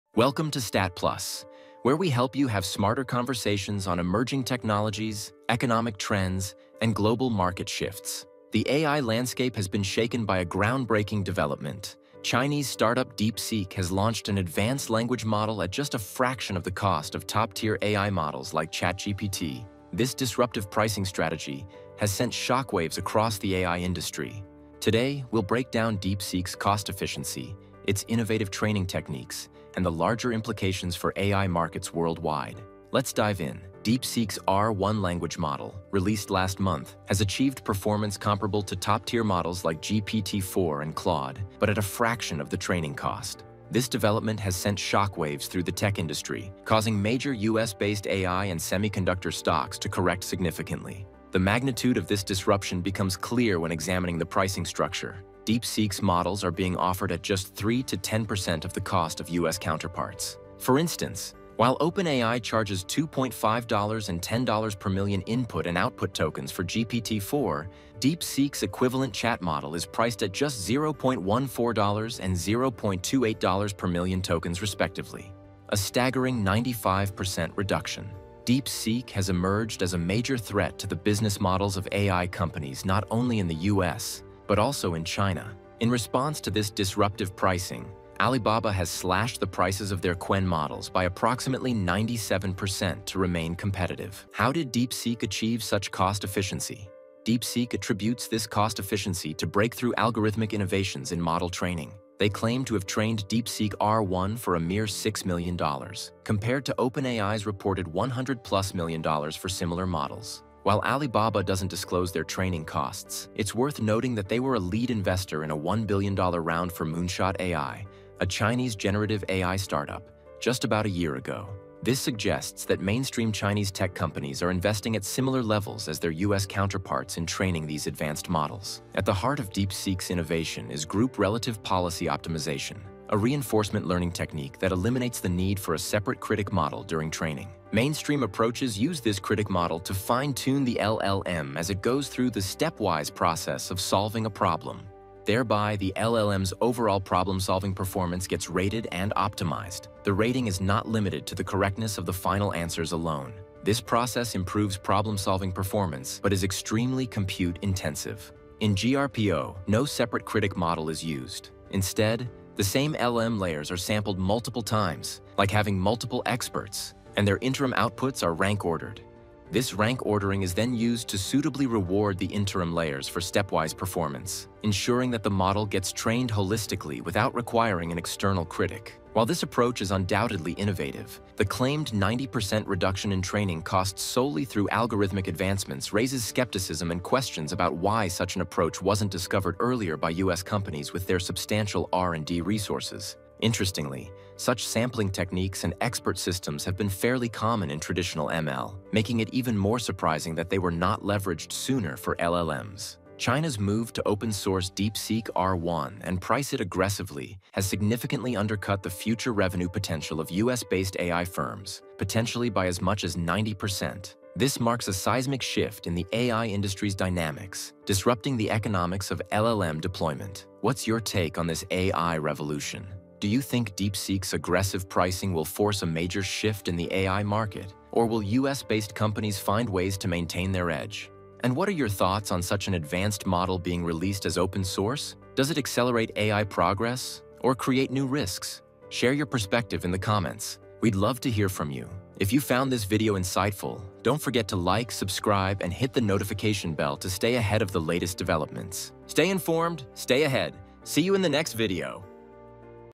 How DeepSeek's $6M Model is Challenging OpenAI’s $100M GPT-4 | Under 5 min
The AI landscape has been shaken by a groundbreaking development—Chinese startup DeepSeek has launched an advanced language model at just a fraction of the cost of top-tier AI models like ChatGPT.

In this video, we’ll break down DeepSeek’s cost efficiency, its innovative training techniques, and the larger implications for AI markets worldwide. Let’s dive in.